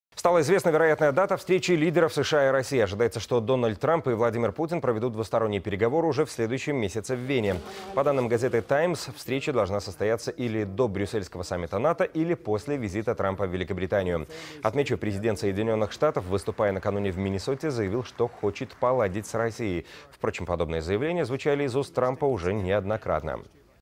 Стала известна вероятная дата встречи лидеров США и России
Ожидается, что Дональд Трамп и Владимир Путин проведут двусторонние переговоры уже в следующем месяце в Вене. По данным газеты "Таймс", встреча должна состояться или до брюссельского саммита НАТО, или после визита Трампа в Великобританию. Отметим, Президент Соединенных Штатов, выступая накануне в Миннесоте, заявил, что хочет поладить с Россией. Впрочем, подобные заявления звучали из уст Трампа уже неоднократно.